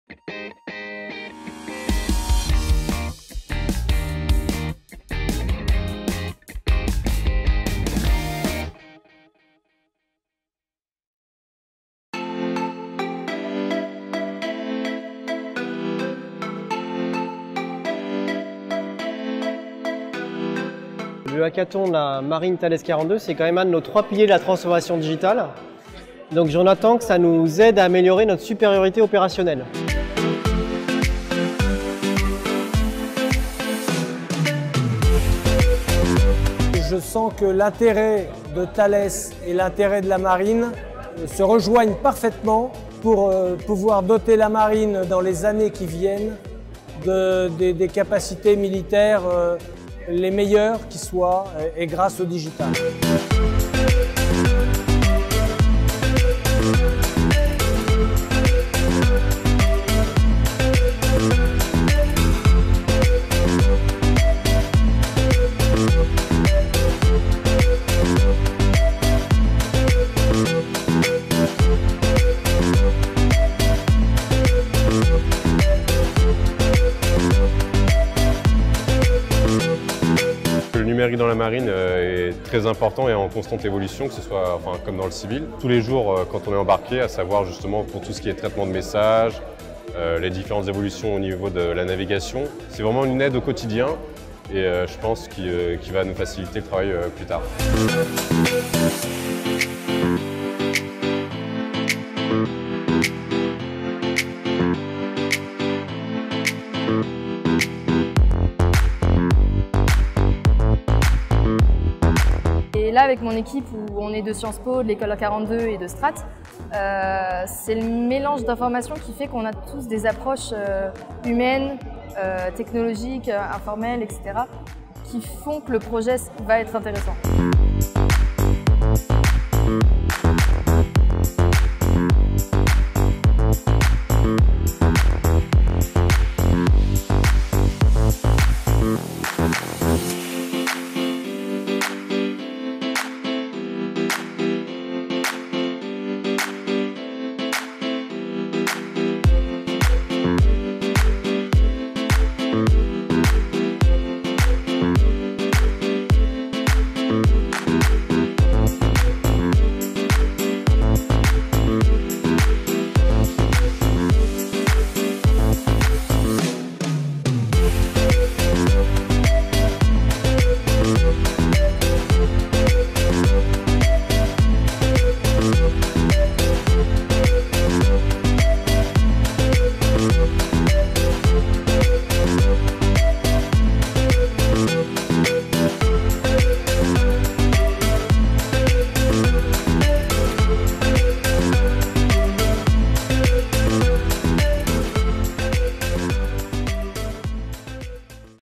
HACKATHON MARINE THALES 2018
Pour la deuxième année, Matrice s'associe à la Marine Nationale et Thales pour un Hackathon dont les projets gagnants seront accompagnés dans leur développement durant 10 mois au sein du programme. Le défi 2018 était de développer un outil
collaboratif de partage destiné à nourrir un cloud de situation maritime entre tous les usagers de la mer.